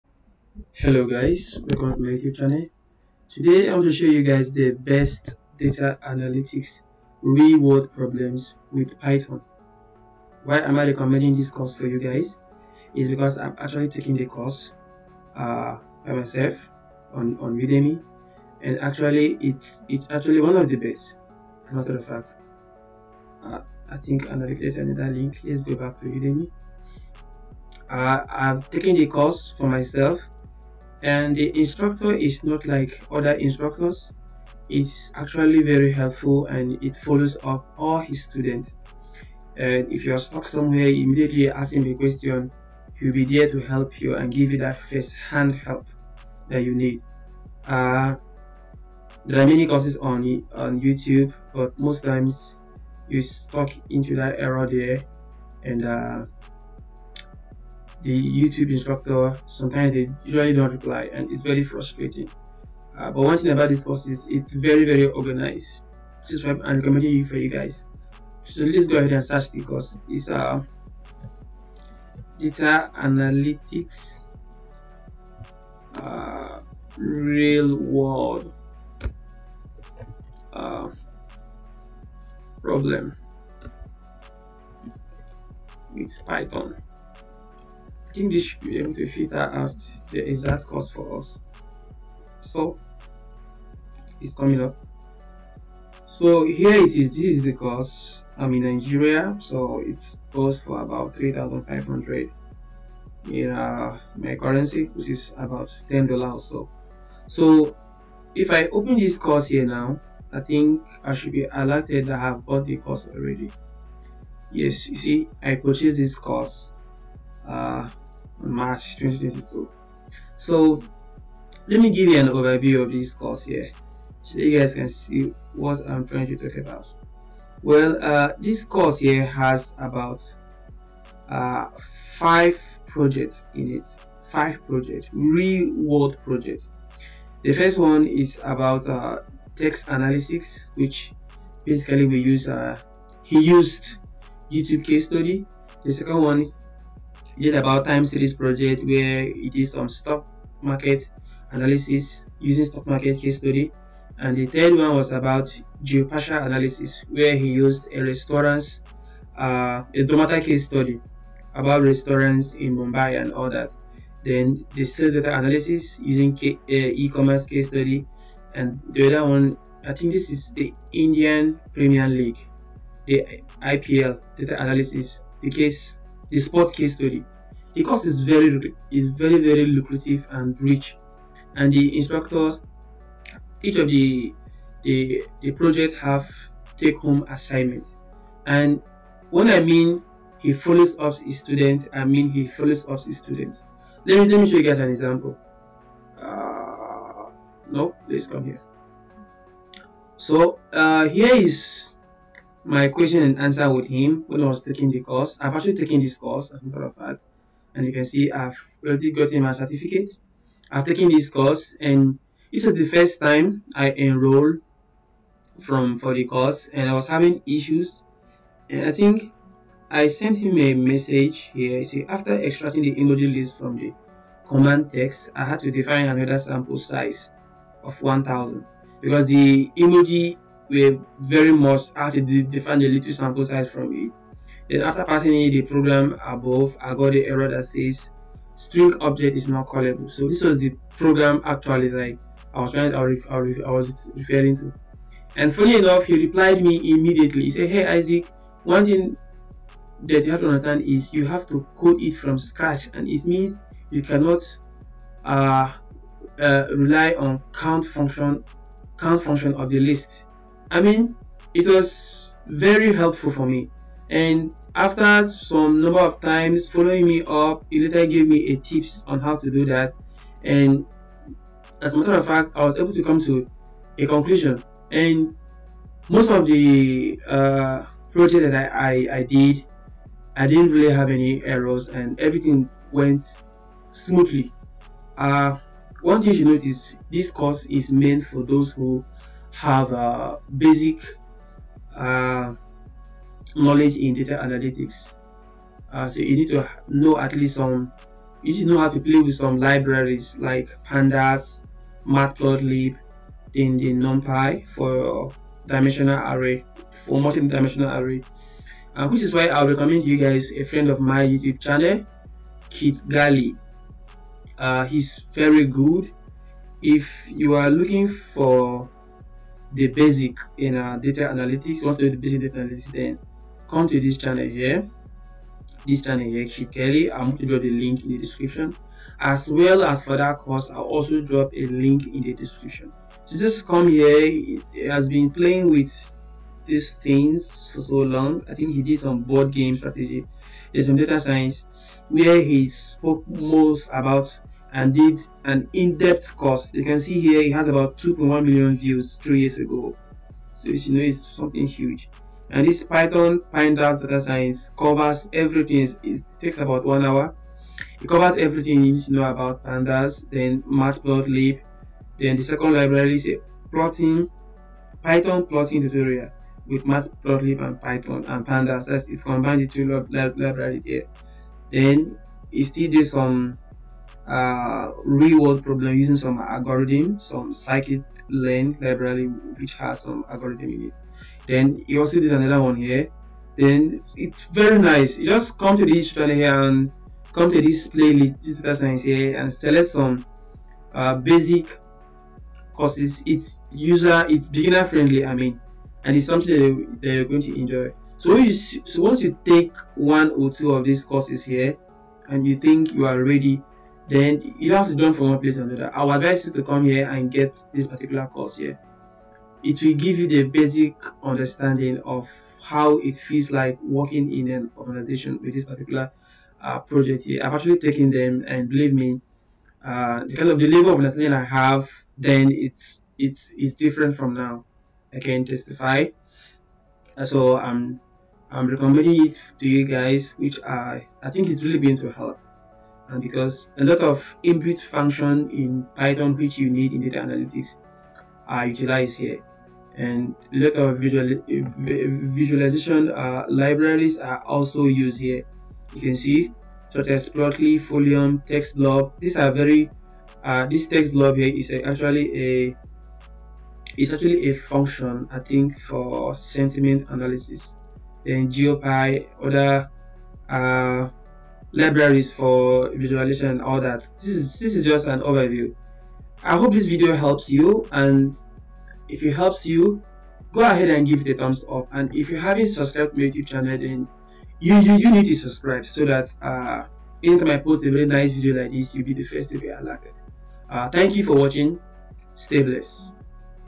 The Best Udemy Course For Data Analytics Real World Problem with Python
In this video, I'll Show you guys the best Best Udemy  Course  For Data Analytics Real World Problem with Python.
The Course Covers 5 Major projects on Text-data,Sales,Time-series,Stock and Sport.

Why should you take this Course?
1. It explains Projects on  real Data and real-world Problems. No toy data! This is the simplest & best way to become a Data Analyst/Data Scientist

2.It shows and explains the full real-world Data. Starting with importing messy data, cleaning data, merging and concatenating data, grouping and aggregating data, Exploratory Data Analysis through to preparing and processing data for Statistics, Machine Learning and Data Presentation.

3.Professionals who are exposed to data but can't yet leverage its power

4.Product managers who want to make data-driven decisions

5.It gives you plenty of opportunities to practice and code on your own. Learning by doing.

6.In real-world projects, coding and the business side of things are equally important. This is probably the only course that teaches both: in-depth Python Coding and Big-Picture Thinking like How you can come up with a conclusion

Guaranteed Satisfaction: Otherwise, get your money back with 30-Days-Money-Back-Guarantee.

Video Chapters:
0:00 Introduction: The Best Python Data Analytics Course on Udemy
2:12 Course Overview and Feedback
5:10 The Best YouTube Chanel for Data Science/Analytics Basics: